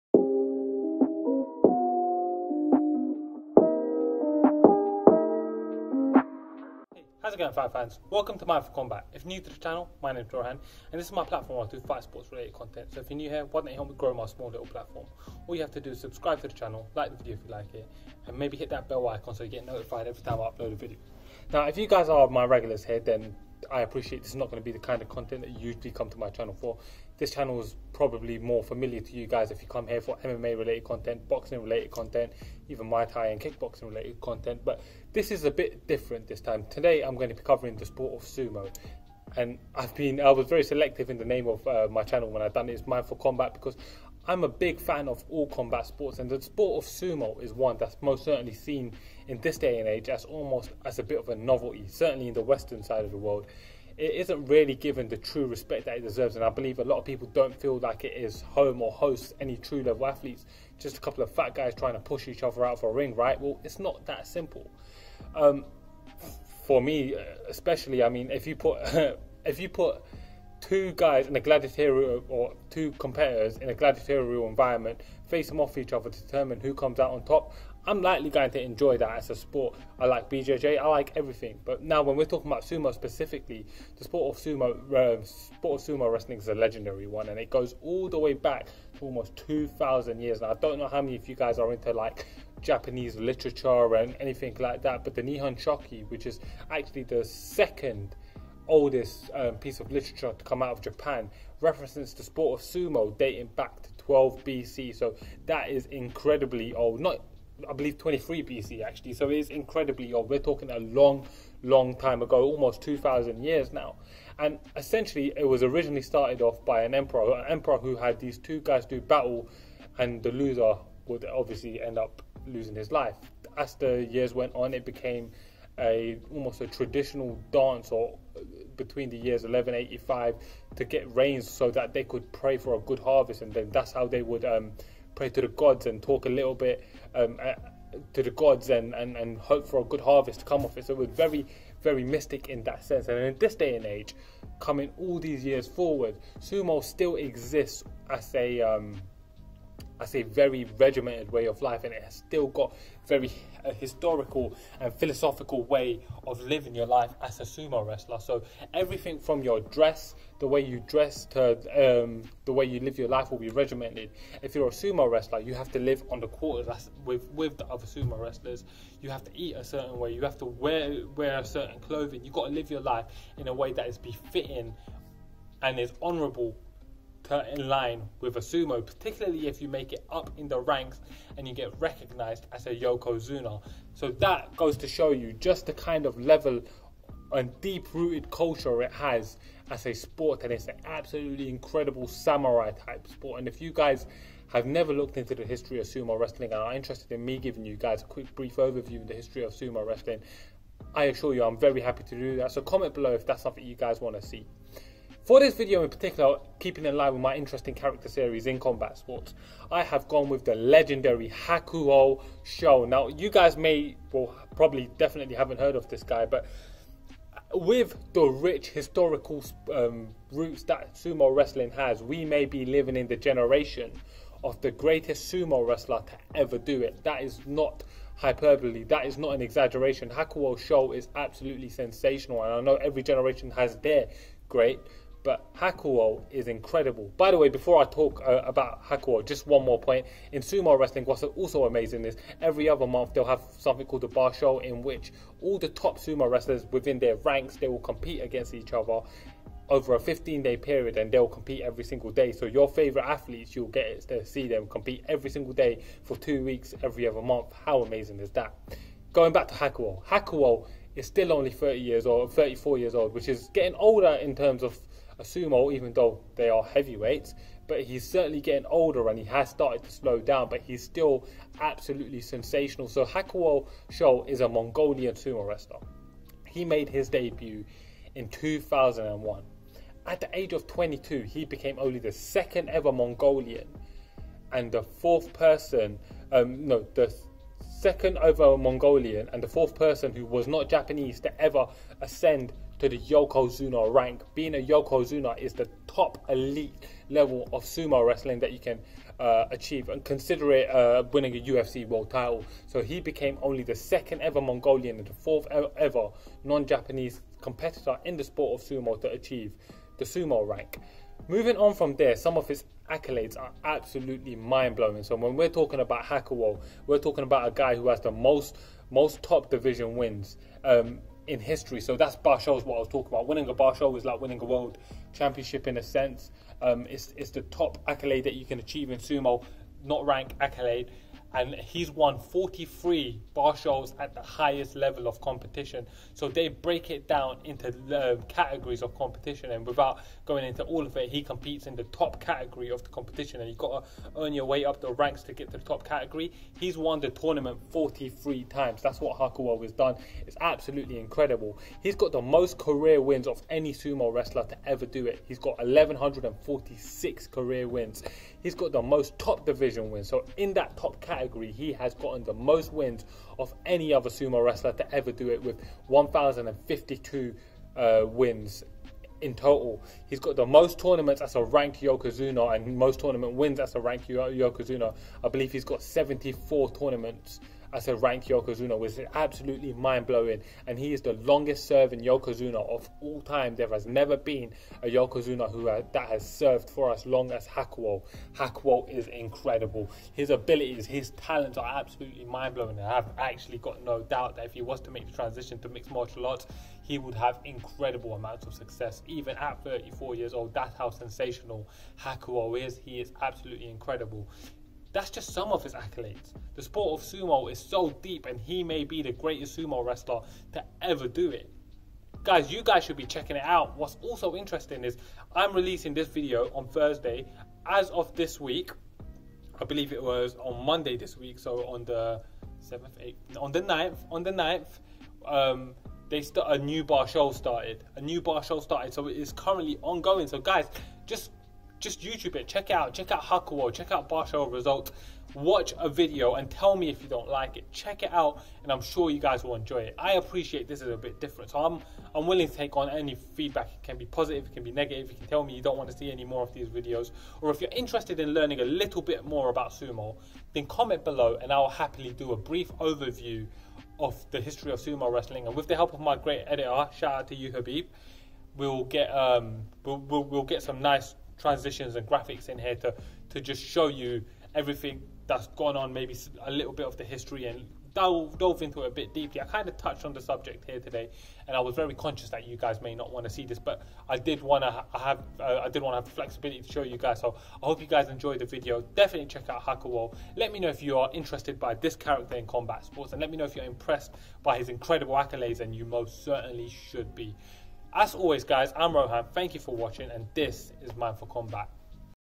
The Legendary Warrior Hakuhō Shō 白鵬翔
Hakuhō Shō is a legendary Sumo Wrestler and in this video I talk about some of his accolades.